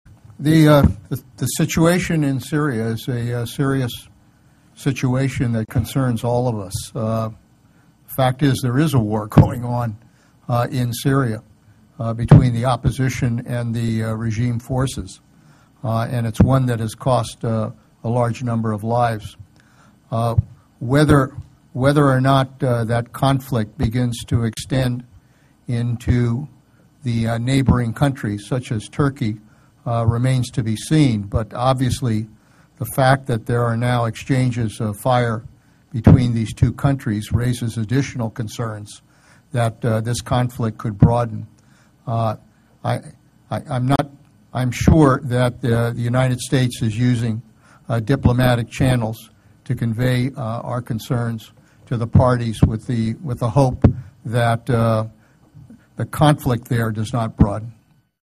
Panetta: Syria Clash With Turkey May Escalate
The continued exchange of artillery fire between Syria and Turkey raises additional concerns that the conflict may escalate and spread to neighboring countries, Defense Secretary Leon Panetta said Saturday.  (Oct. 6)